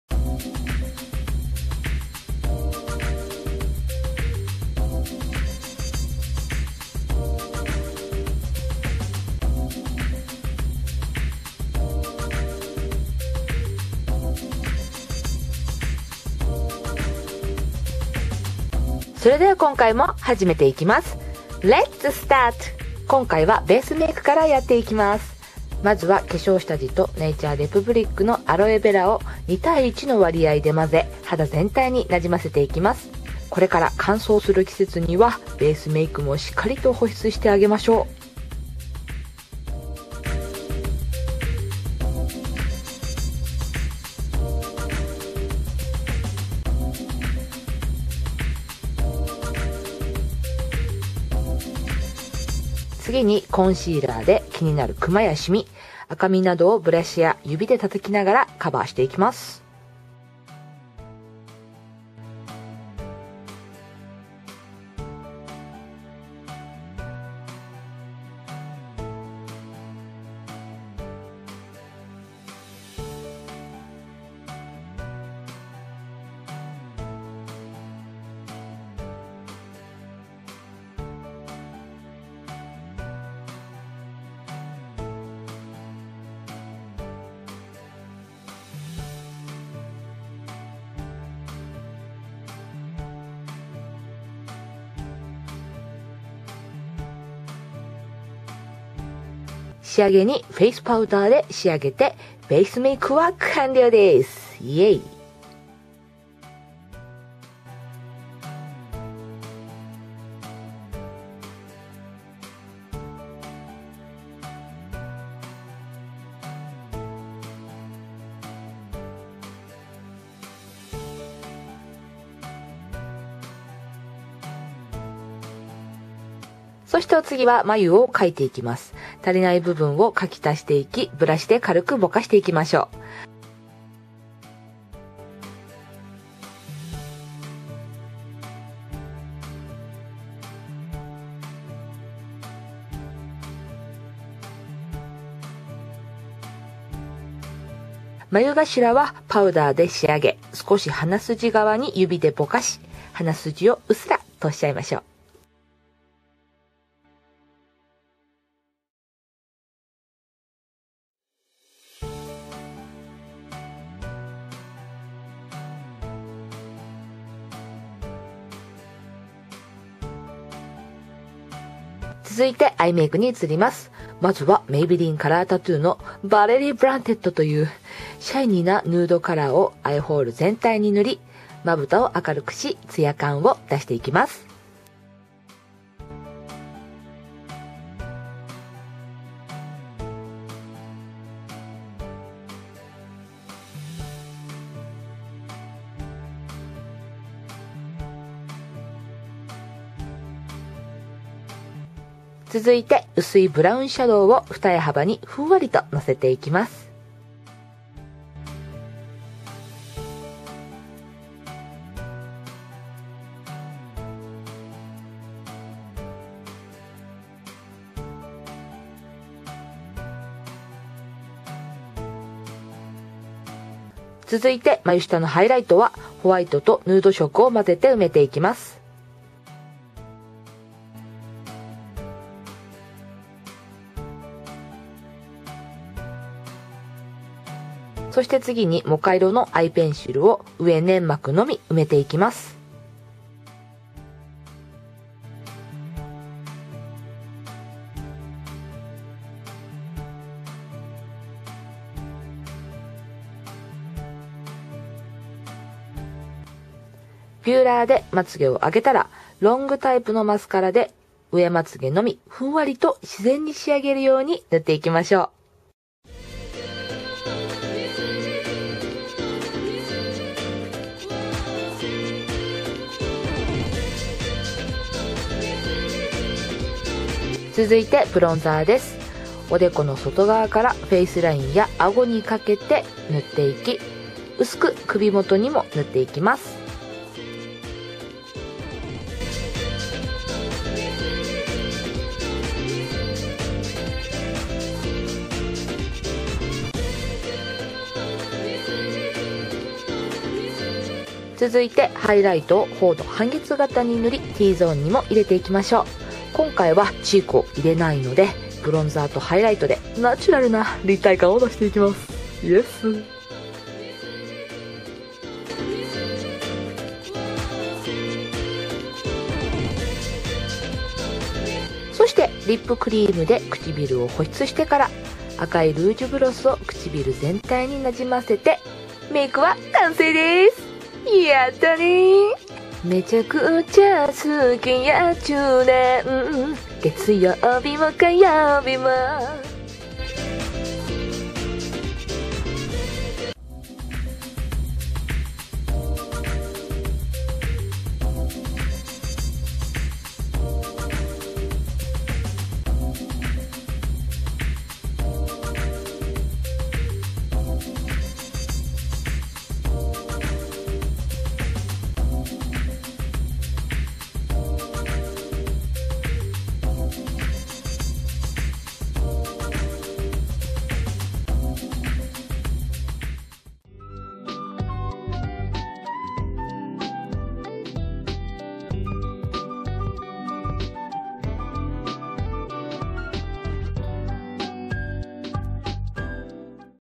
Winter Makeup Shiny Natural 冬の保湿ツヤナチュラルメイク
はるにゃんです！
これから乾燥する季節なので！
ベースメイクからの～自然なツヤ感とシャイニーな
ナチュラルメイクです☆
チークは今回はなし！！！